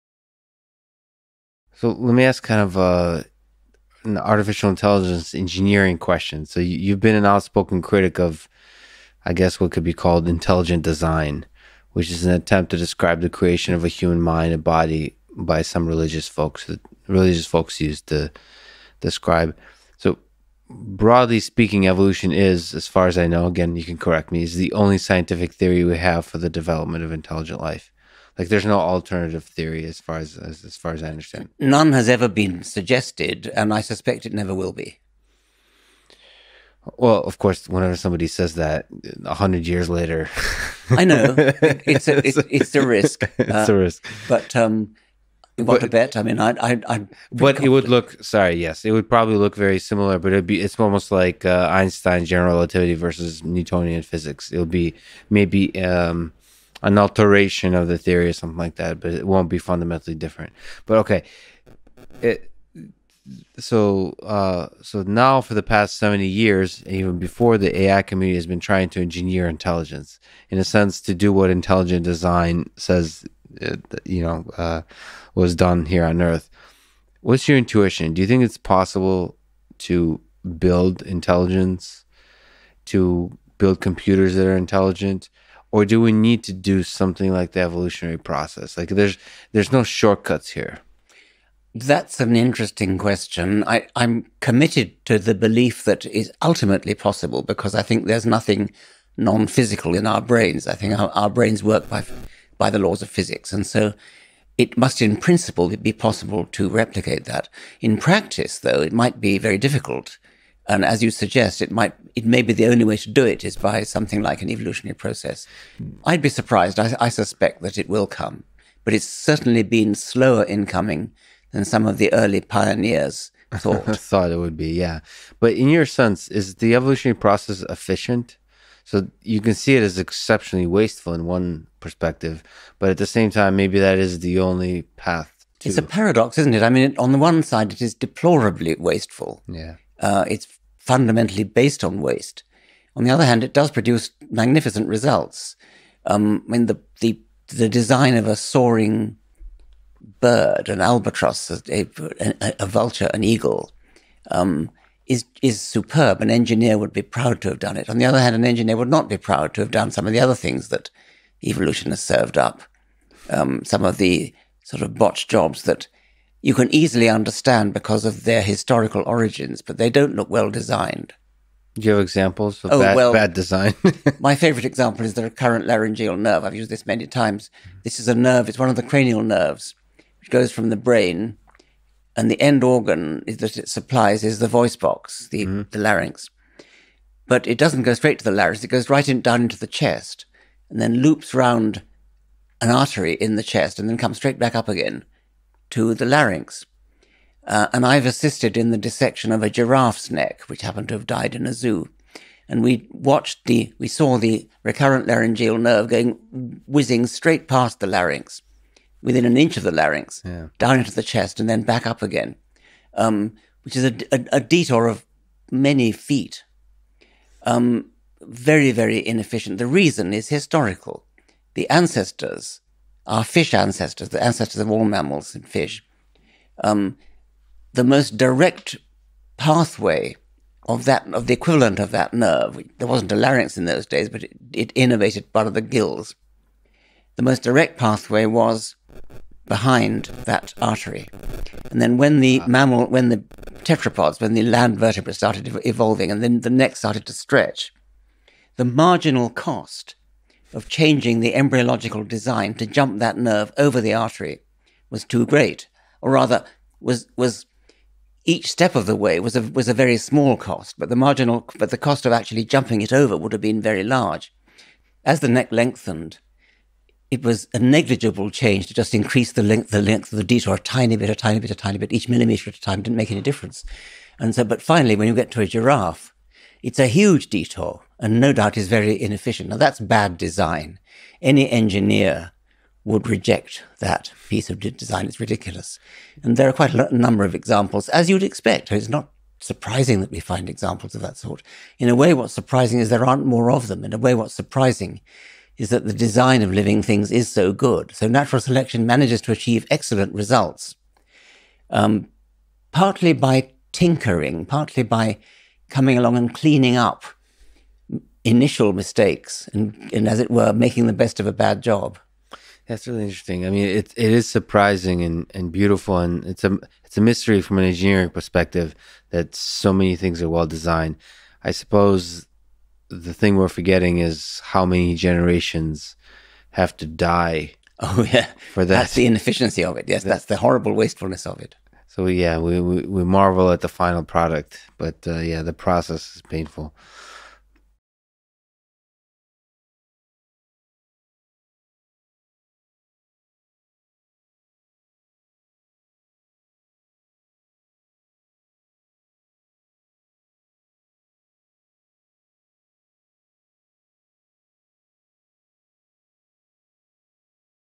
Is there a shortcut to evolution in engineering intelligence? (Richard Dawkins) | AI Podcast Clips
Richard Dawkins is an evolutionary biologist, and author of The Selfish Gene, The Blind Watchmaker, The God Delusion, The Magic of Reality, The Greatest Show on Earth, and his latest Outgrowing God. He is the originator and popularizer of a lot of fascinating ideas in evolutionary biology and science in general, including funny enough the introduction of the word meme in his 1976 book The Selfish Gene, which in the context of a gene-centered view of evolution is an exceptionally powerful idea. He is outspoken, bold, and often fearless in his defense of science and reason, and in this way, is one of the most influential thinkers of our time.